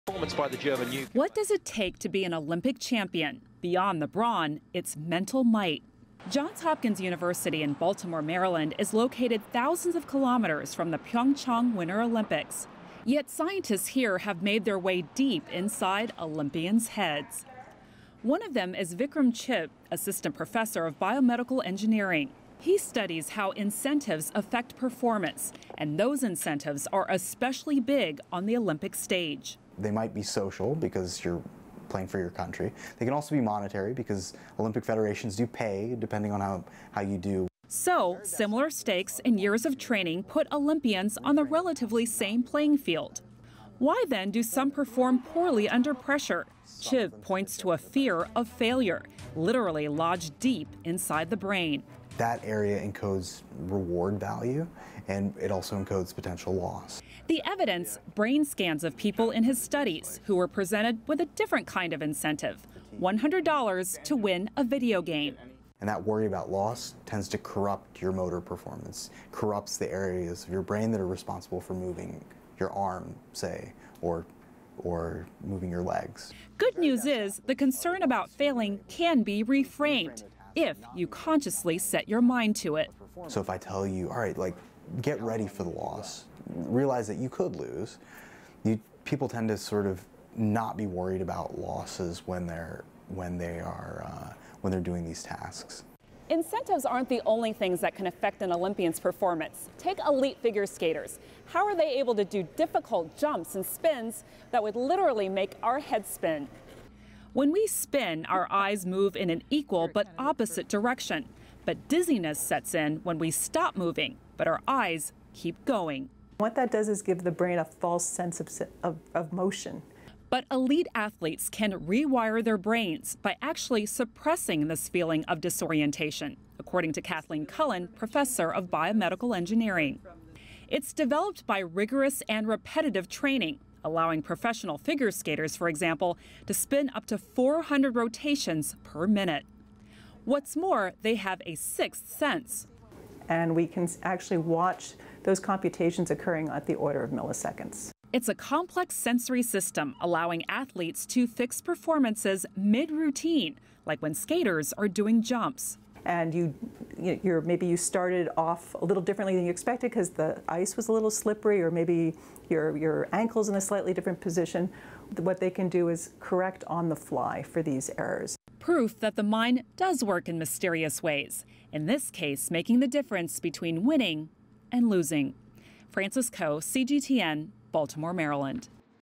Scientists research how the brain molds Olympic champions
What does it take to be an Olympic-level athlete? Beyond the brawn, it’s mental might. CGTN’s Frances Kuo explains.